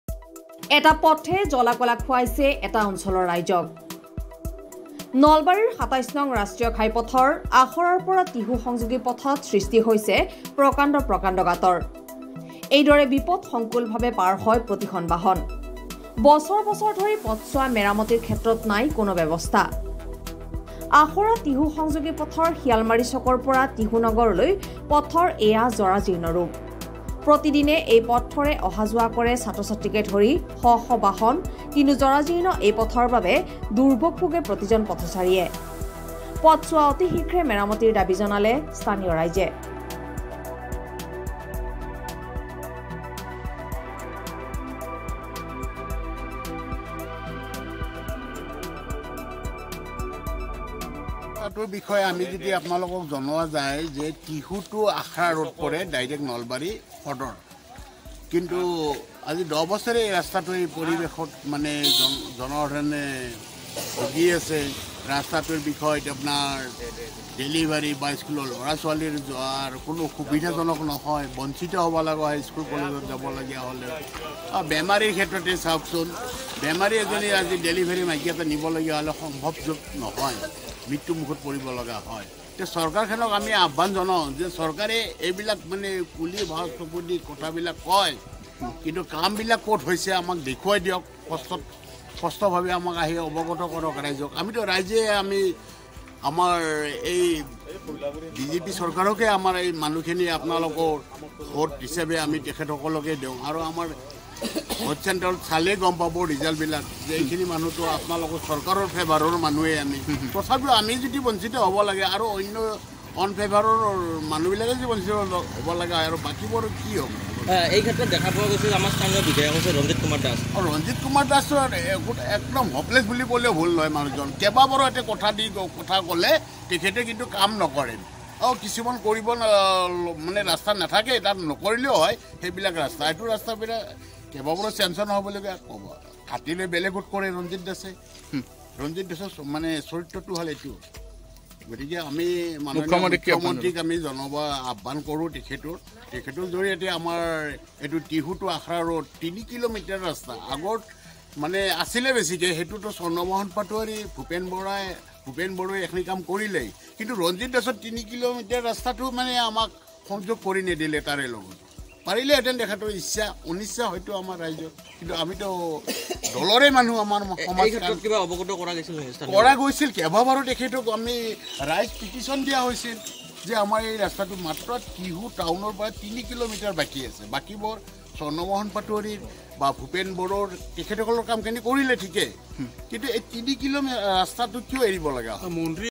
এটা পথে জ্বলাকলা খুৱাইছে এটা অঞ্চলৰ ৰাইজক
নলবাৰীৰ ২৭ নং ৰাষ্ট্ৰীয় ঘাইপথৰ আখৰাৰ পৰা টিহু সংযোগী পথৰ দুৰৱস্থা